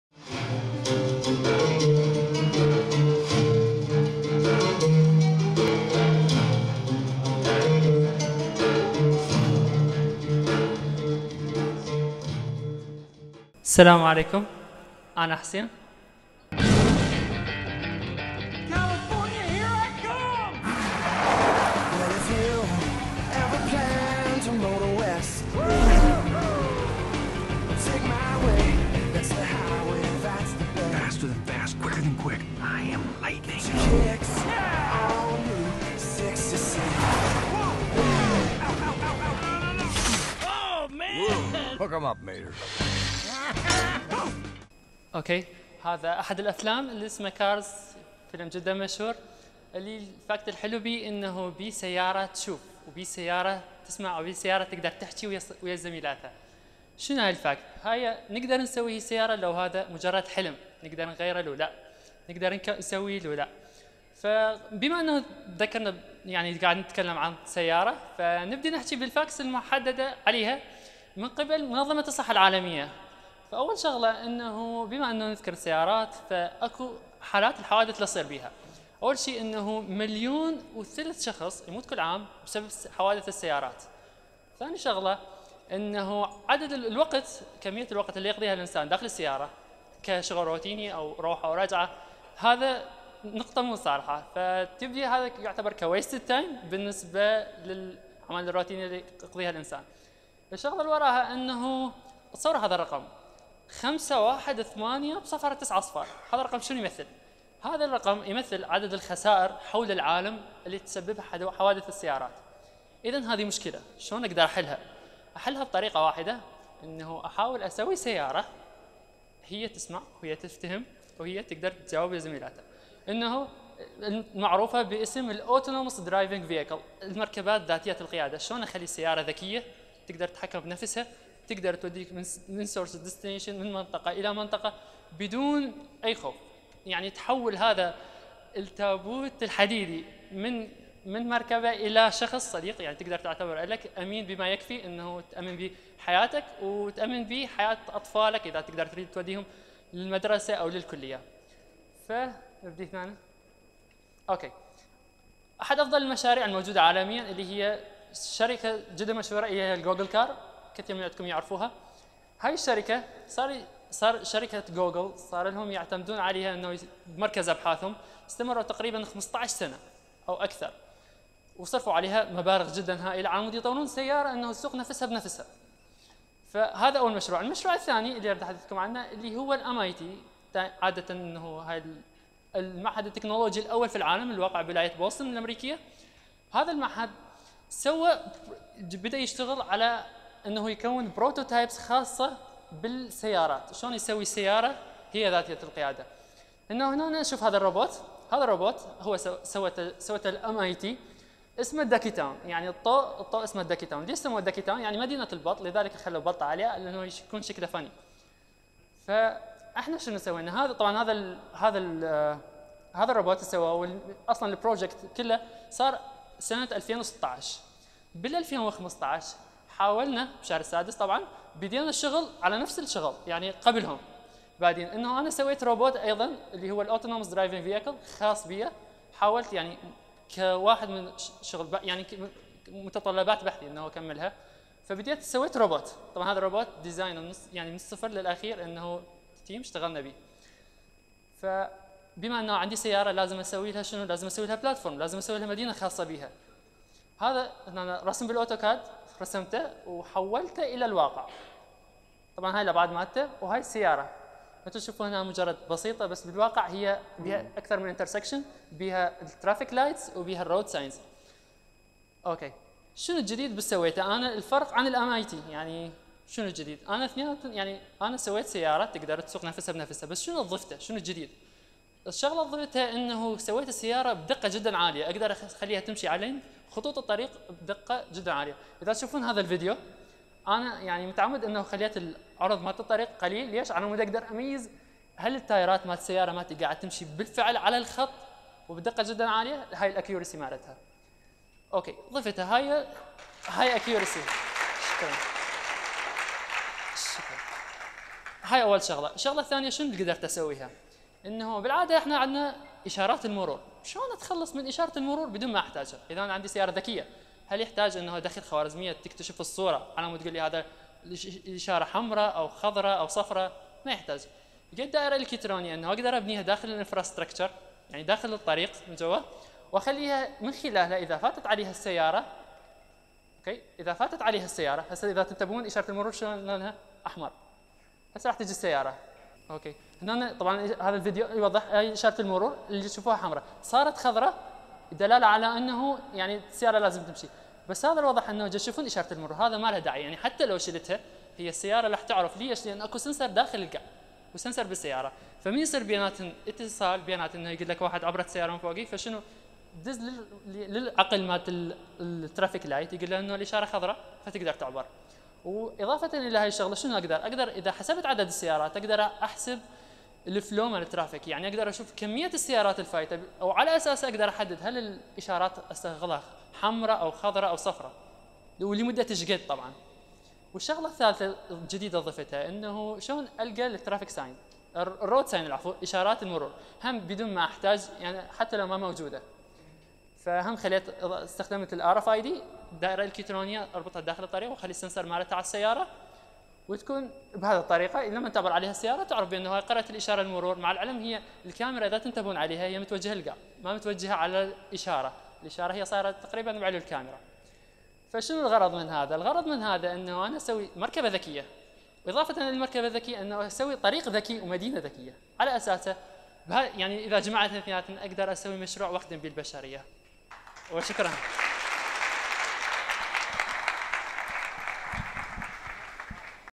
Autonomous Driving Vehicles in Smart Cities | Hussein Al-Ghanim | TEDxManawiBasha
Hussein is talking now about his project in converting vehicles to be self-driving and the way to achieve this by his own studies, which began since 2015.

Born in Basra 1992, studied in its University, and graduated from Computer Engineering department with honor degree. He is about to get his master of science in “Autonomous driving vehicles”.
He participated and published few papers in international conferences.
He works as a lecturer in Basra gifted school and provides gifted individuals with support in programming and scientific consultancy . His mission is to spread scientific knowlege and to contribute in developing and innovating scientific programs and applying them in robotics